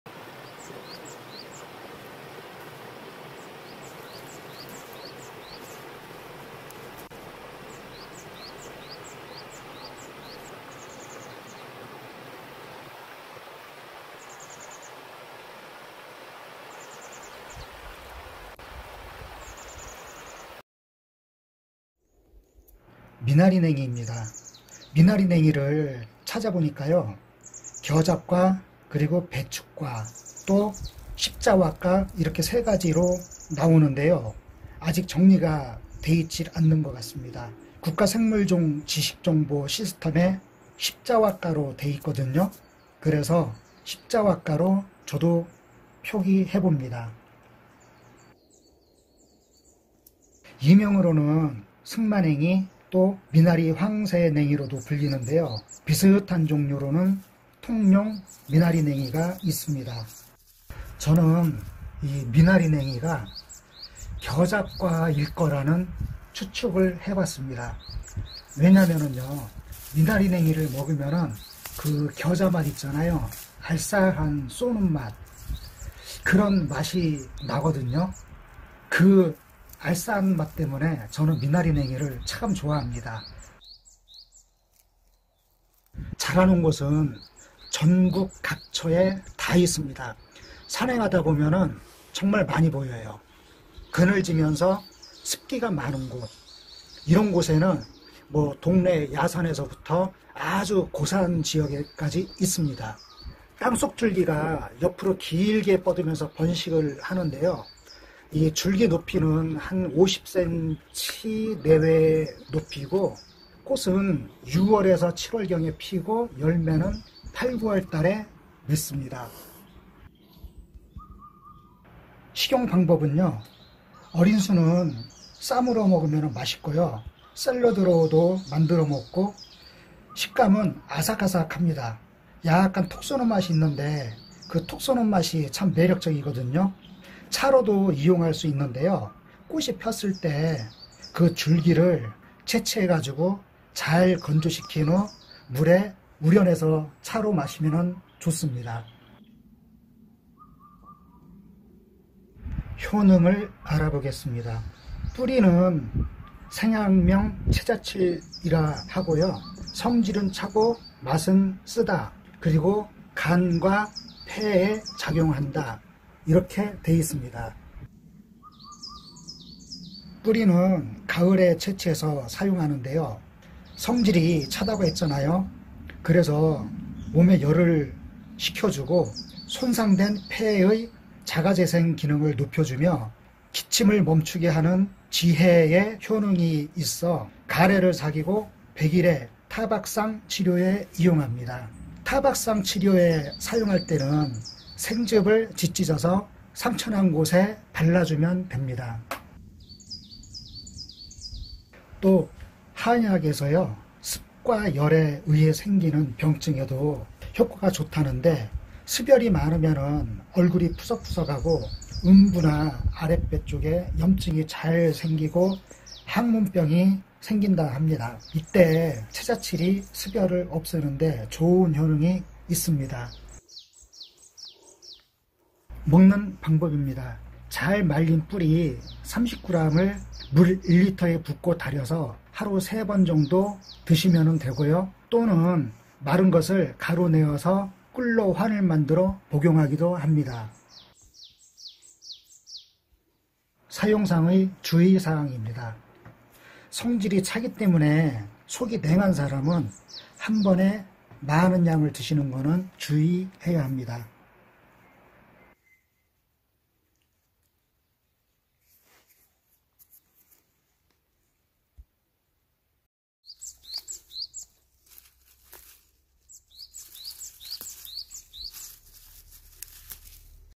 미나리냉이/ 폐 세포를 재생시켜 주는 놀라운 효능...폐기능 향상시키고 기침 가래 습열로 인한 두통 얼굴 푸석거림 음부 가려움증에 효능
흔하면서도 맛있는 산나물 이 호흡기에도 좋고
가침.가래.타박상.백일해 에 이용.